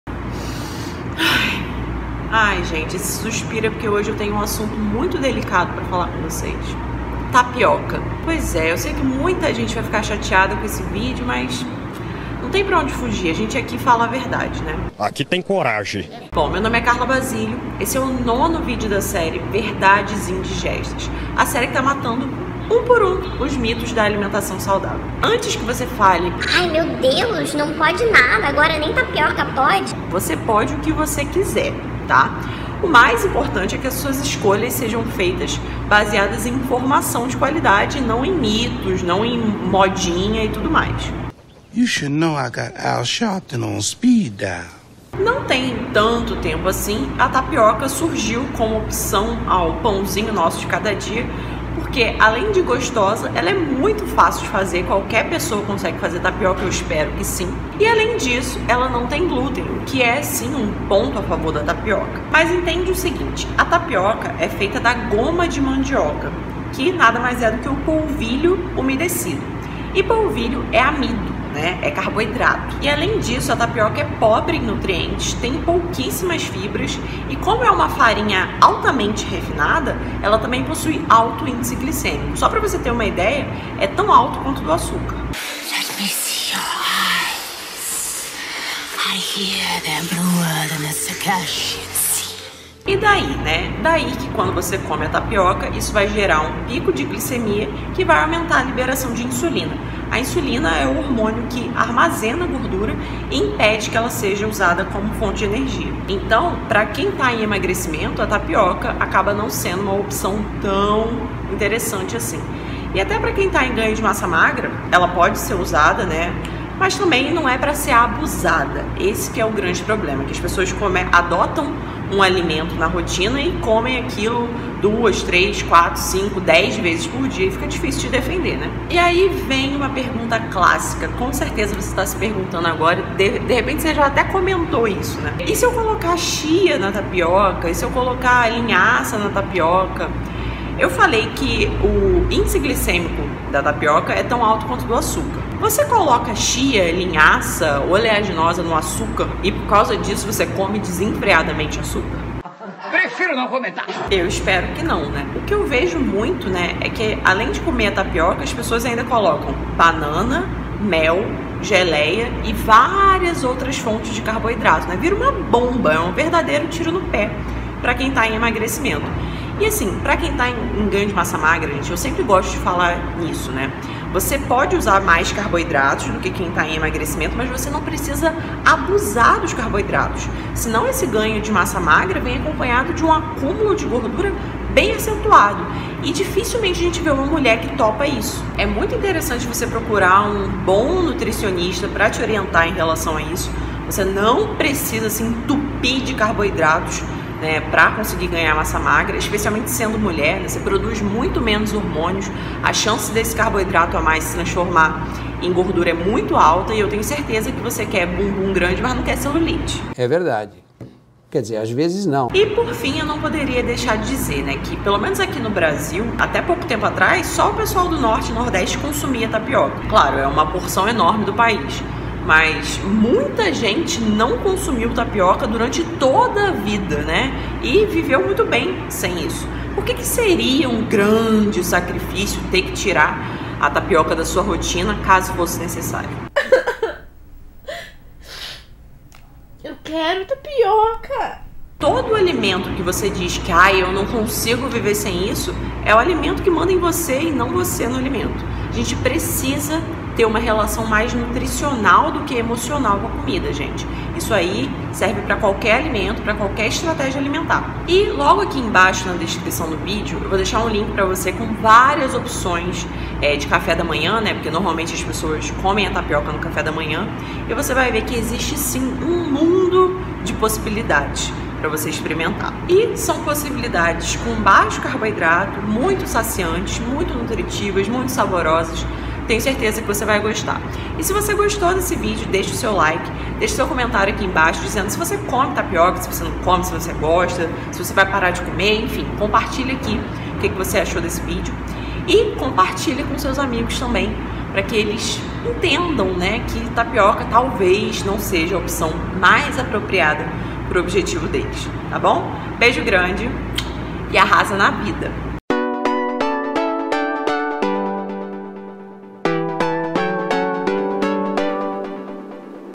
MITOS E VERDADES SOBRE A TAPIOCA NA DIETA | Verdades Indigestas - EP09 | CARLA BASÍLIO
Saiba os mitos e verdades sobre a 'queridinha' das dietas, que conquistou uma posição de destaque no cardápio de quem faz dieta. Assista agora!

Produção:
Gostart Digital